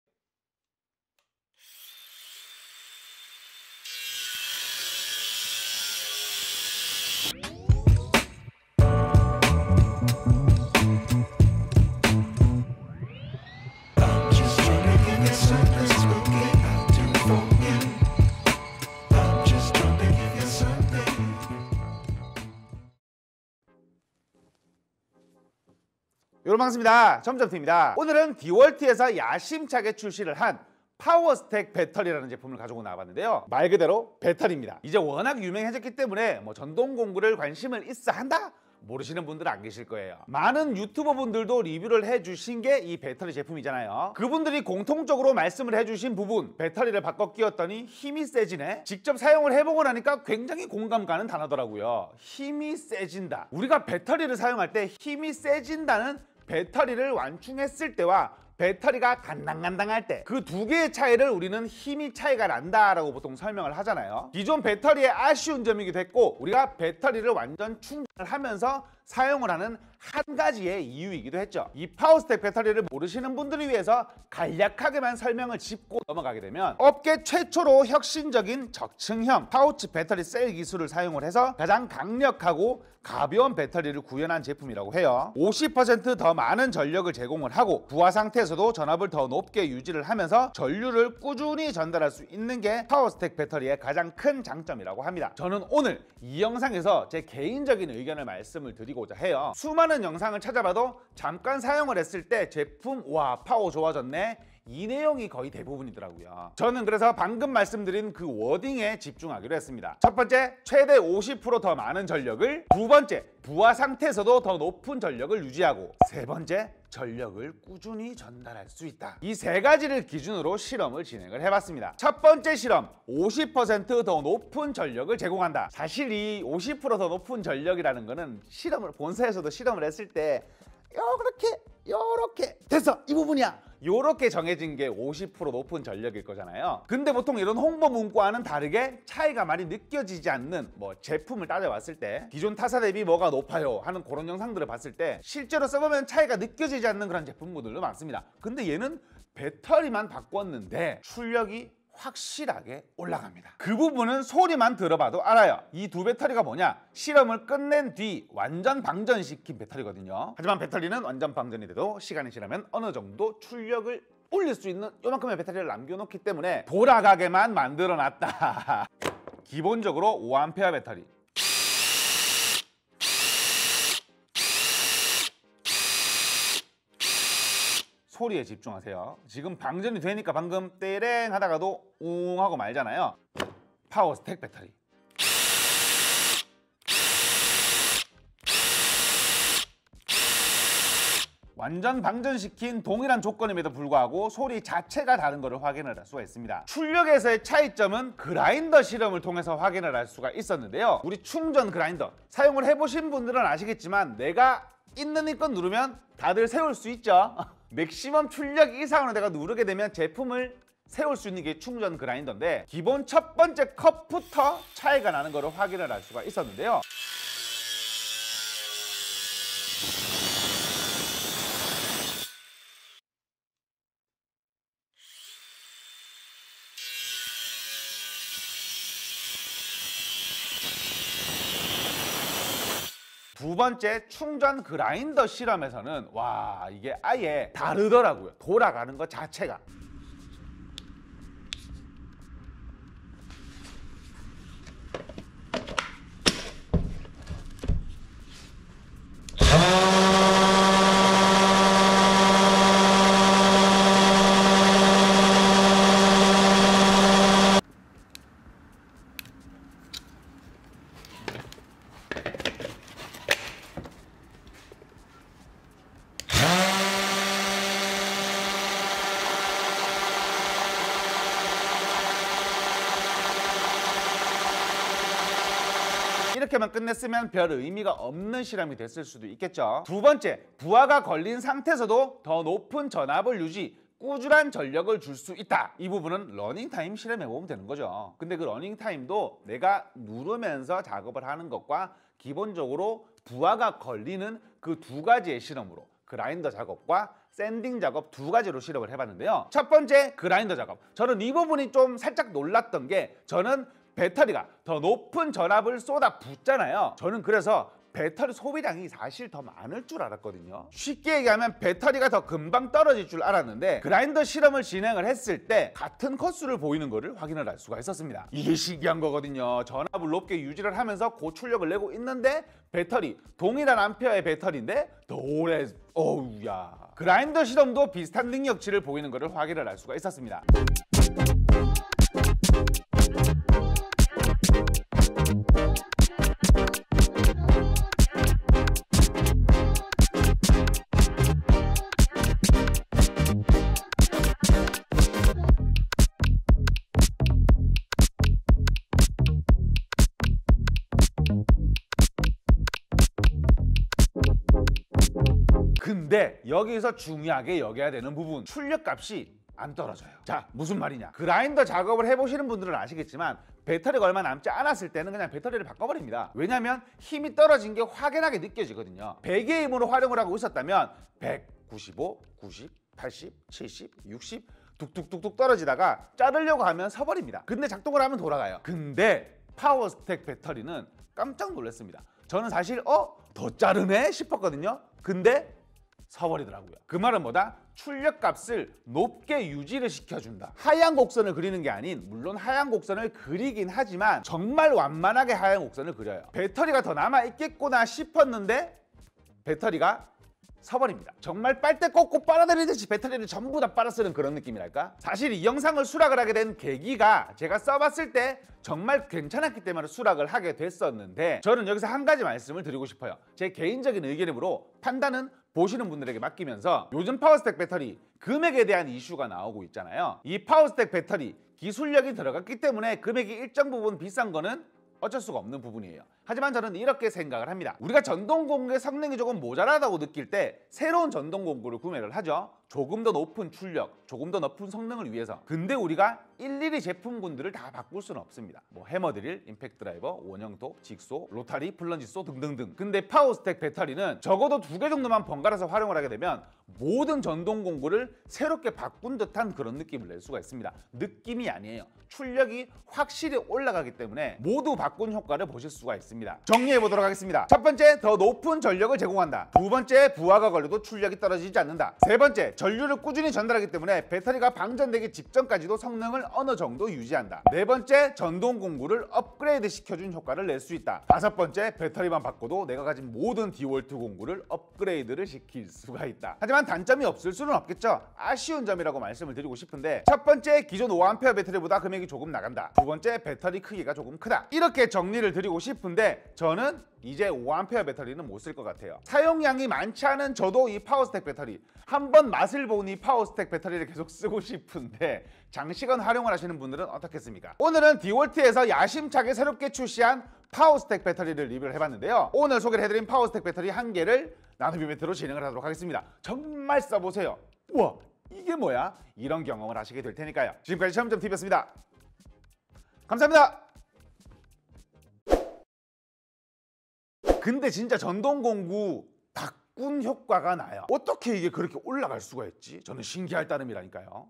모든제품을 강제로 성능 업그레이드시켜준다더니 뭐..지? 헐.. [디월트 파워스택베터리 DEWALT POWERSTACK BATTERY]
순간적인 파워보다도 그파워가 일정하게 끝까지간다는거에
더큰 점수를 주고싶은 베터립니다. 금액아 내려가라아아아ㅜㅜ

*인스타그램 팔로우해주신분들 사랑합니다